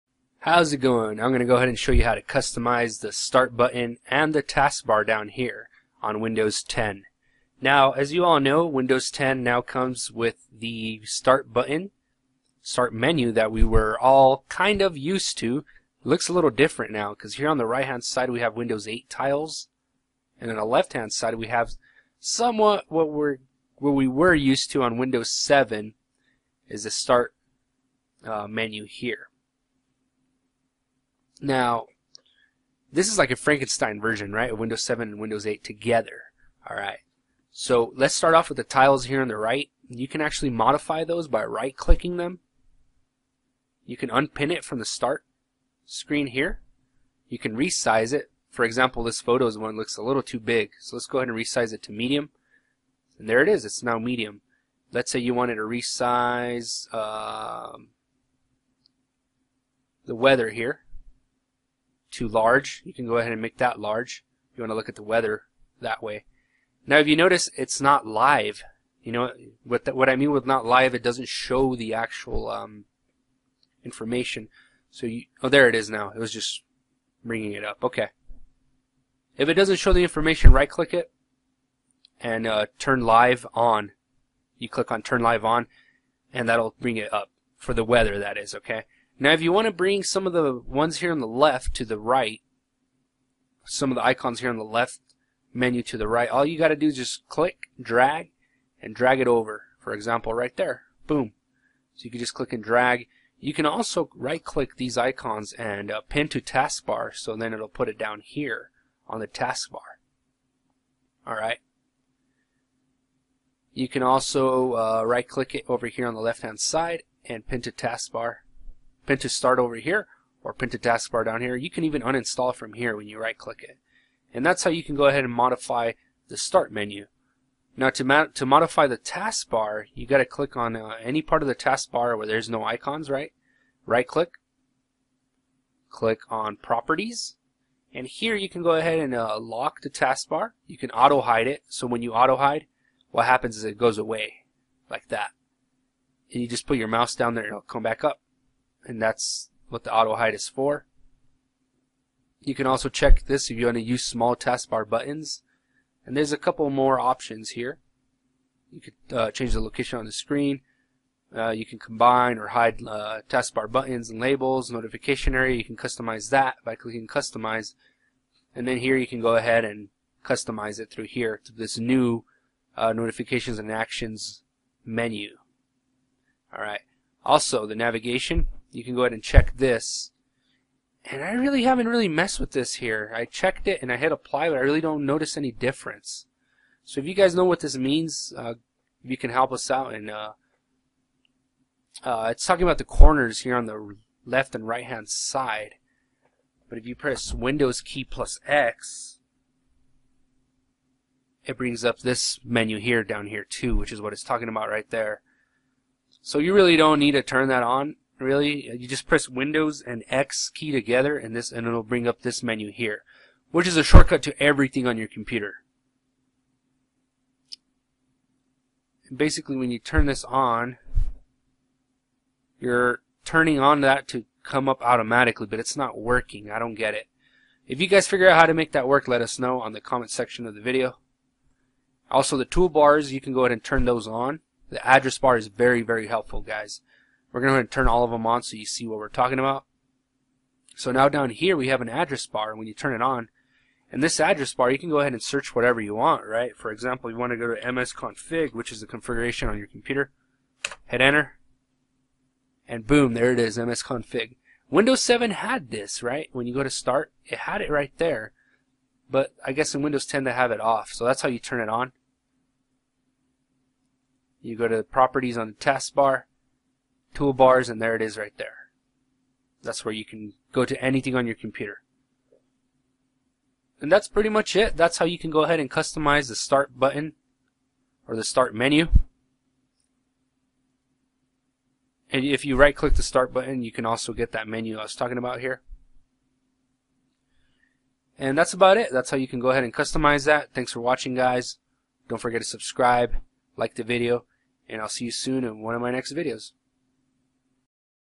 How To Customize The Start Menu and Taskbar On Windows 10
In this video I Will Show You How To Customize The Start Menu and Taskbar On Windows 10

. __________Recommended Software_____________

tune-up.